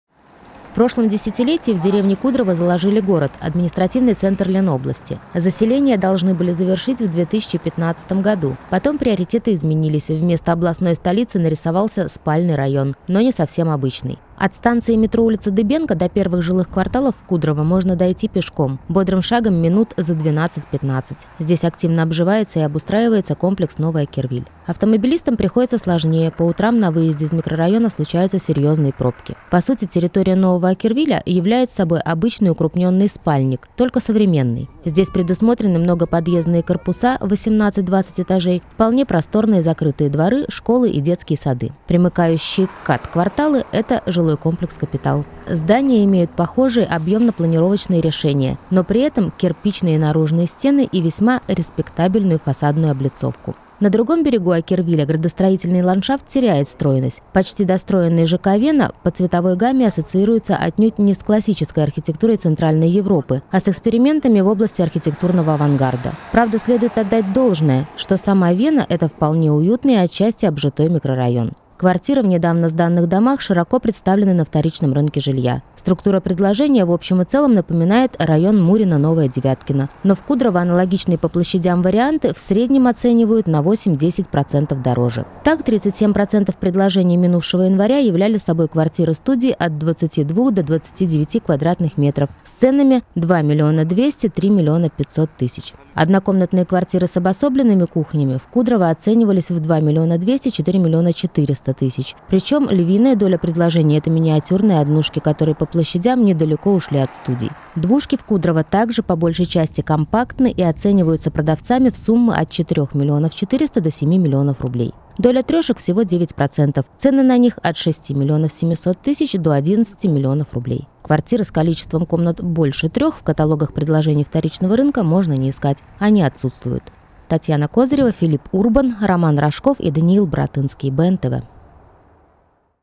Новая вторичка Кудрово: два берега Оккервиля
В прошлом десятилетии в деревне Кудрово заложили город – административный центр Ленобласти. Заселение должны были завершить в 2015 году. Потом приоритеты изменились, и вместо областной столицы здесь вырос не совсем обычный спальный район.